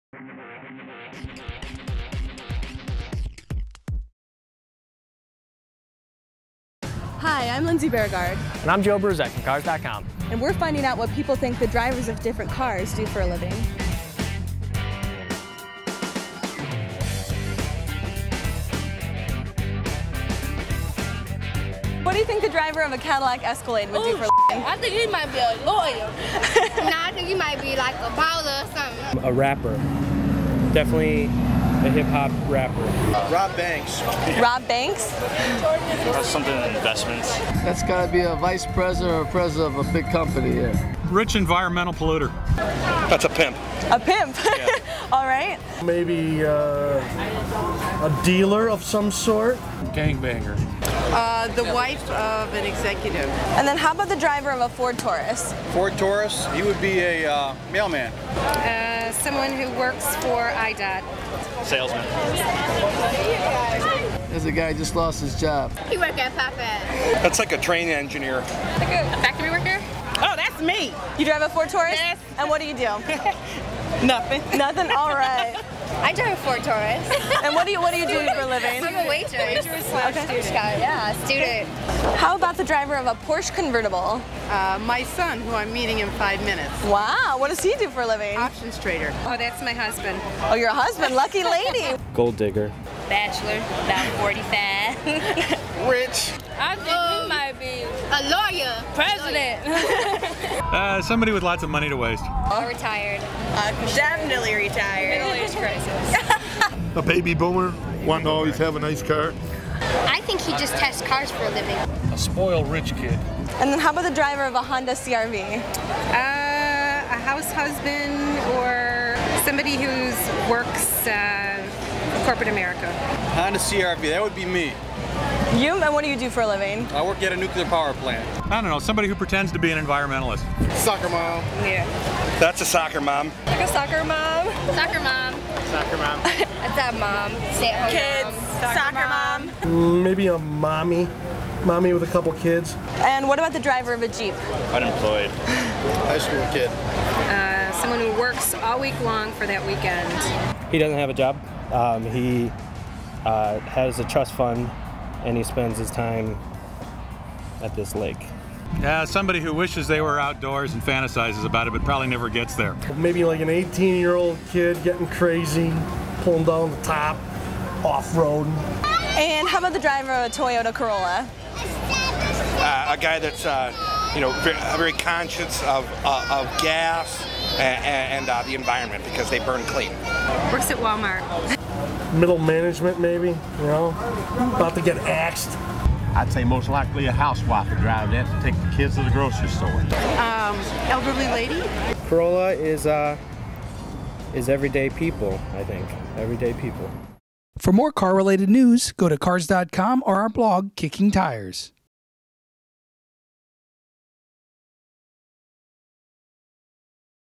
Career Cars
Cars.com's Lindsay Bjerregaard and Joe Bruzek hit the streets to see what people think the drivers of different cars do for a living.

Shot and produced by:  Eric Rossi, Sarah Gersh, and Matthew Avery
Edited by:  Lindsay Bjerregaard